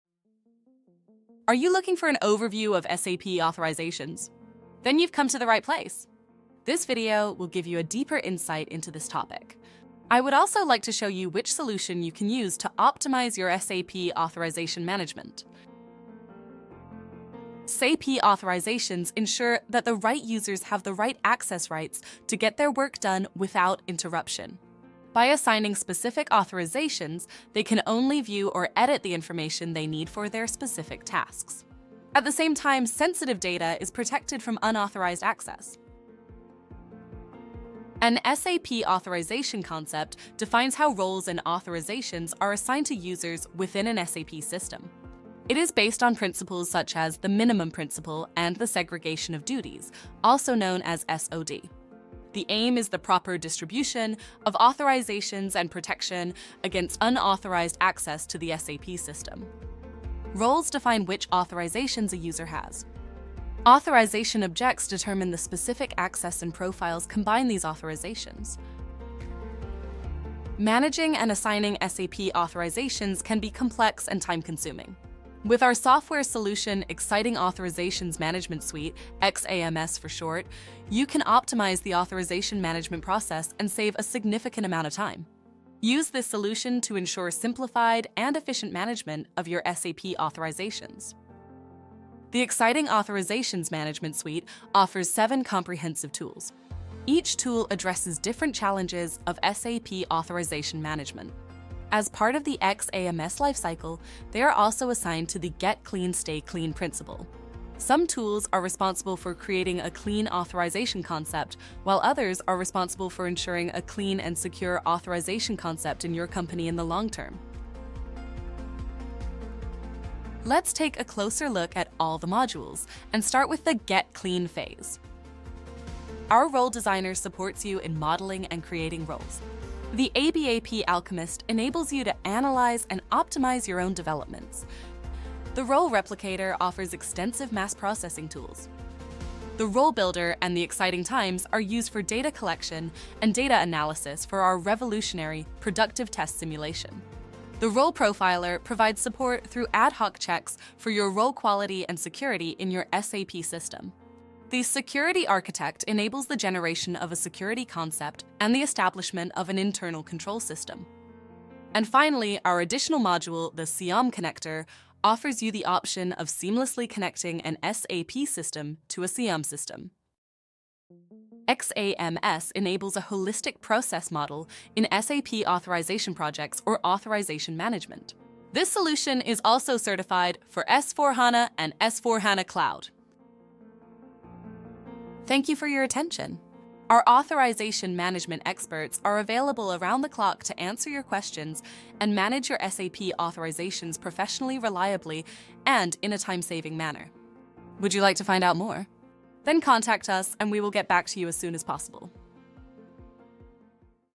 What are SAP authorizations?
Are you looking to understand SAP authorizations in depth? This video provides an overview of SAP authorization management, including key concepts like user roles, authorization objects, and profiles that help protect sensitive data and ensure only the right users have access to the necessary information.

Discover how SAP authorization concepts, based on principles like the minimum principle and segregation of duties, can optimize security and efficiency. We also introduce the powerful Xiting Authorizations Management Suite (XAMS), designed to streamline and simplify your authorization management process with seven specialized tools. These tools cover everything from role design and development optimization to security architecture and SIEM integration, offering a holistic solution for SAP authorization projects, certified for both S/4HANA and S/4HANA Cloud.

Watch to learn how XAMS’ “Get Clean, Stay Clean” principle can revolutionize your SAP authorization workflow, saving time and boosting security. Ready to enhance your SAP authorization management? Contact our experts anytime for tailored support.